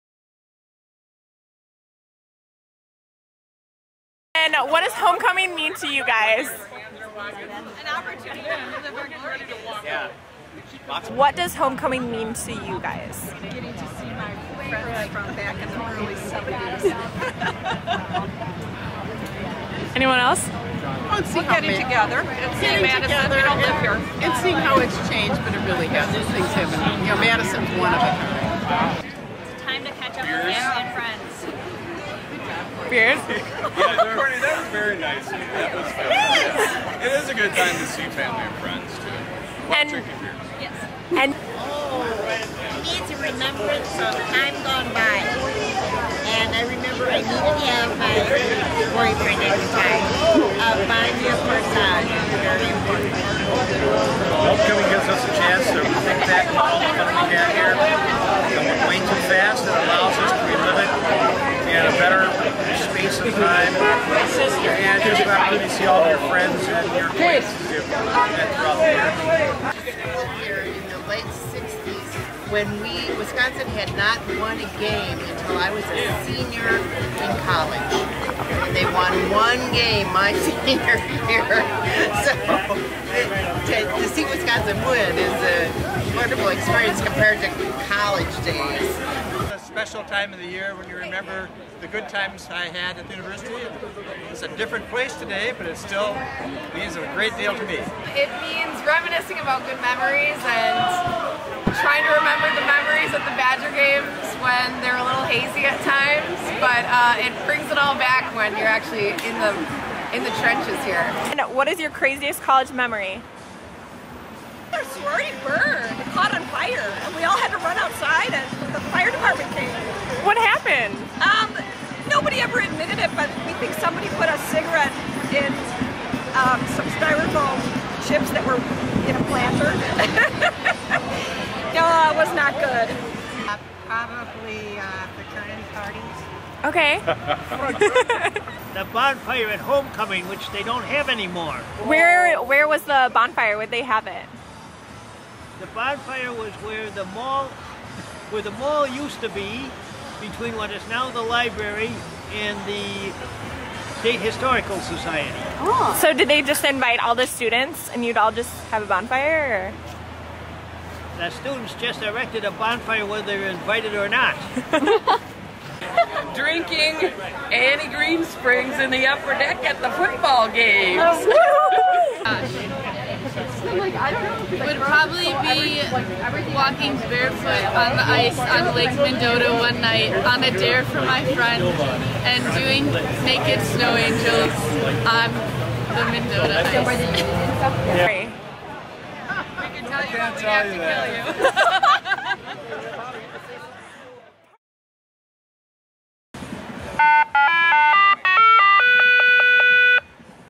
Wisconsin Homecoming 2013
The Daily Cardinal captured the spirit of Homecoming 2013. In this video, Alumni of all ages reflect on what Homecoming means to them, as well as share some of their wildest Madison memories.